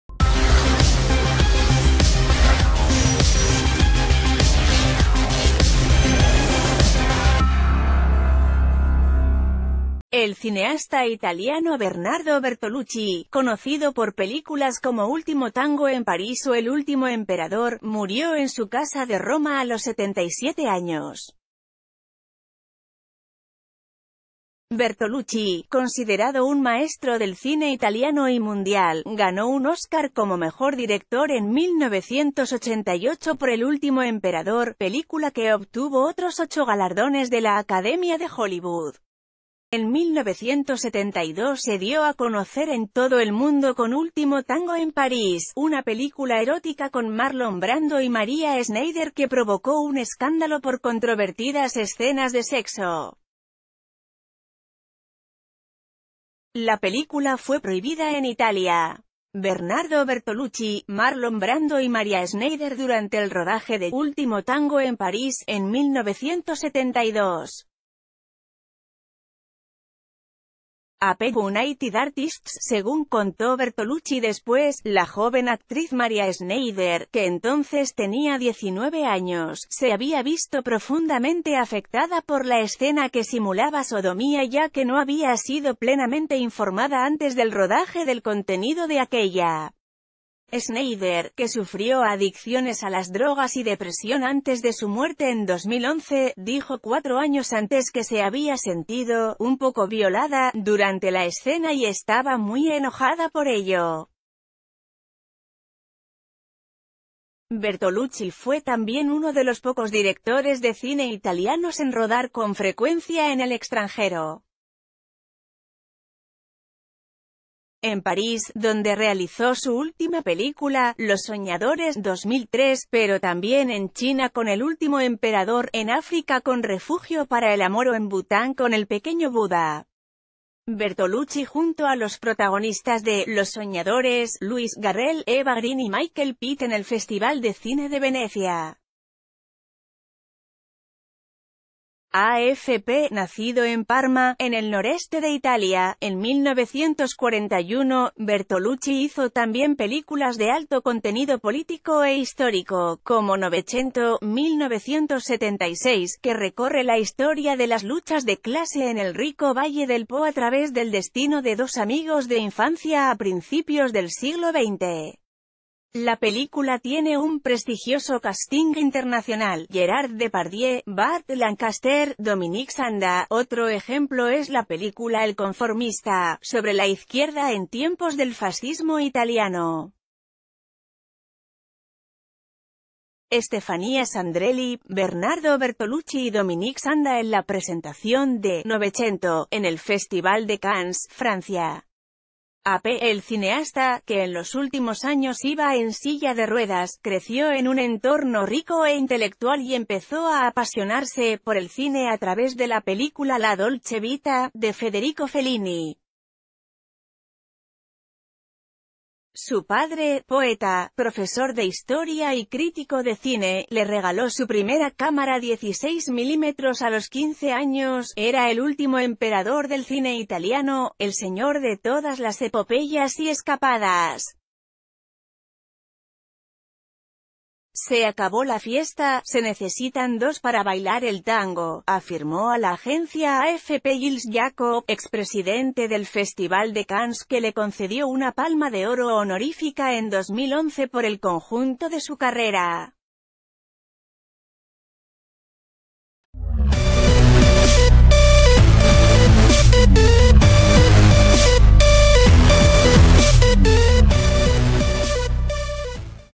Murió Bernardo Bertolucci, el director de "Último tango en París"
El cineasta italiano Bernardo Bertolucci, conocido por películas como Último tango en París o El último emperador, murió en su casa de Roma a los 77 años. Bertolucci, considerado un maestro del cine italiano y mundial, ganó un Oscar como mejor director en 1988 por El último emperador, película que o...